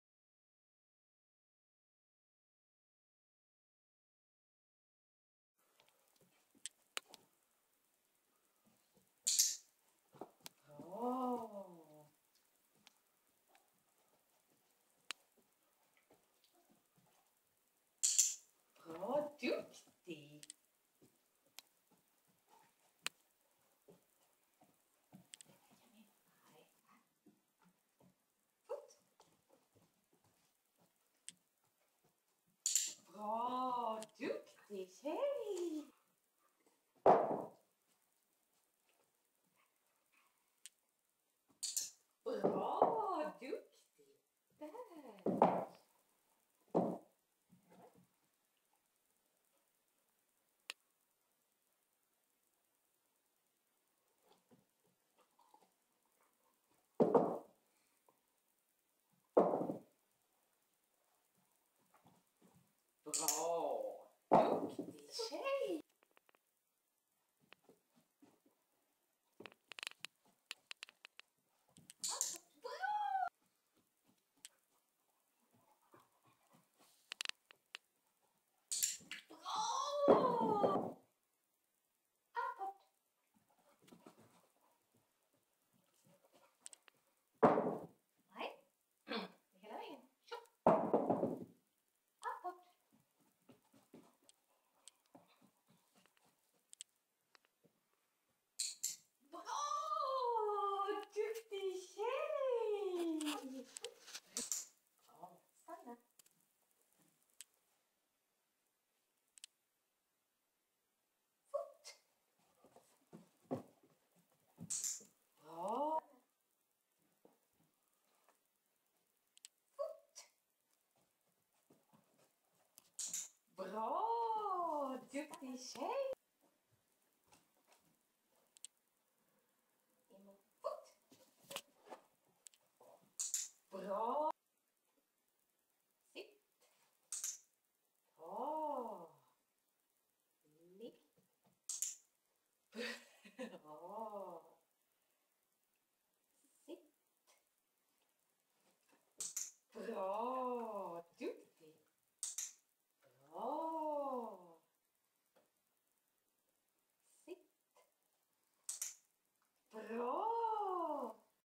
Imola 20131104 Film 1
Resultat efter 2 månader Operant inlärning. Imola Briard 5 månader